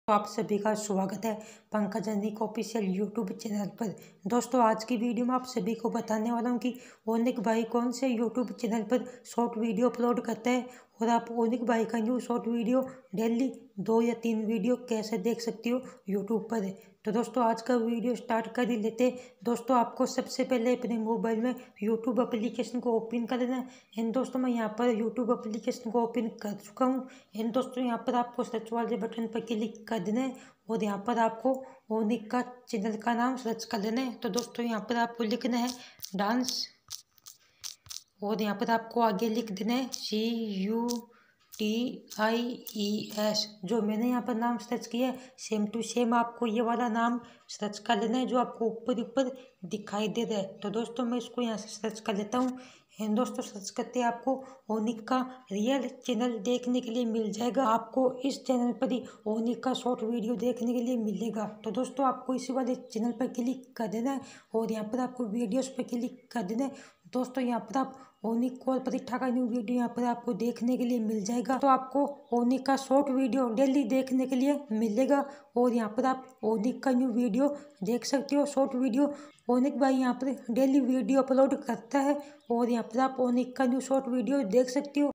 Anik,s Konse Youtube Channel Par Shorts Videos Uplode Karta Hai
Topic coverd:-

1. Anik pritha ka shorts video kaise dekhe

2. Anik ka shorts videos ka channel konsa hai